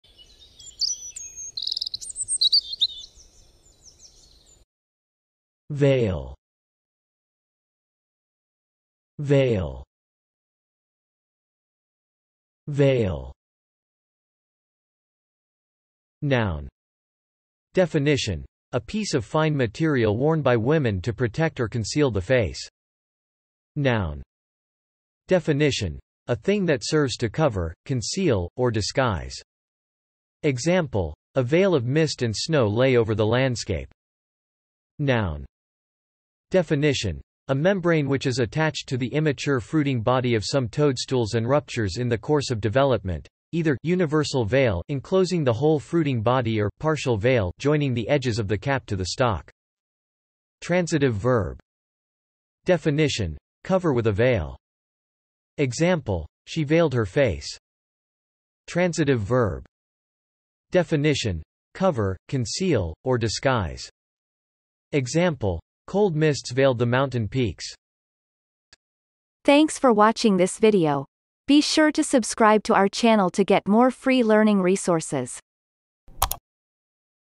How to pronounce veil - Vocab Today
The pronunciation of the word veil in Amercian accent is demonstrated in this video. If the word is from another language, such as brand name, it will be pronounced in its original language. Based on the word frequency, this is of 3 stars difficulty.

The word veil has the following meanings:
1. (noun)
Definition : A piece of fine material worn by women to protect or conceal the face.

2. (noun)
Definition : A thing that serves to cover, conceal, or disguise.
Example : a veil of mist and snow lay over the landscape
3. (noun)
Definition : A membrane which is attached to the immature fruiting body of some toadstools and ruptures in the course of development, either (universal veil) enclosing the whole fruiting body or (partial veil) joining the edges of the cap to the stalk.

4. (transitive verb)
Definition : Cover with a veil.
Example : she veiled her face
5. (transitive verb)
Definition : Cover, conceal, or disguise.
Example : cold mists veiled the mountain peaks